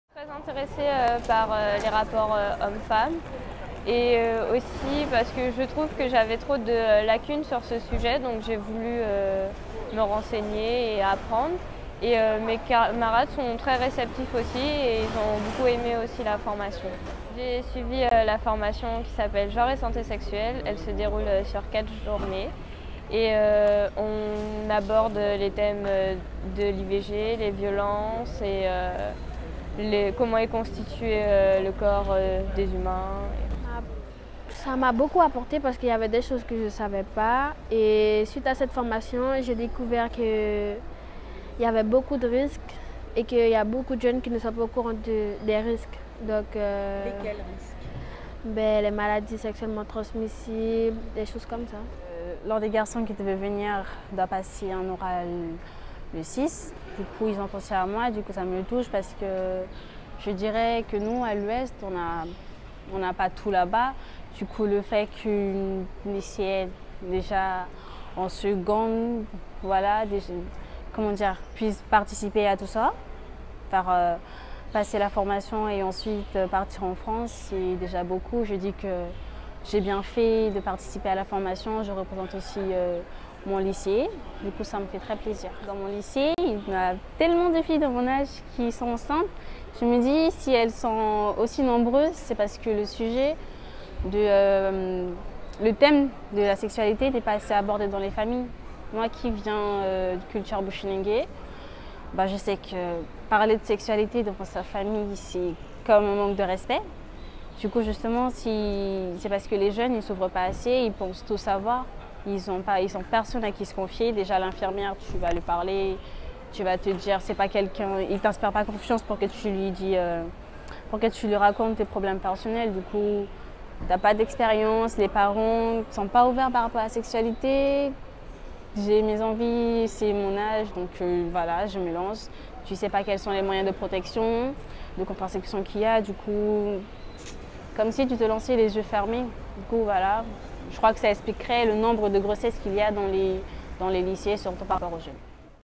Les ambassadrices Egalité filles-garçons - Guyane 1ère
Iris, Sylvan et Nadichah ambassadrices Egalité filles-garçons vont faire un partage d'expérience au ministère de la santé et à l'université du planning familial à Rennes du 7 au 9 juin.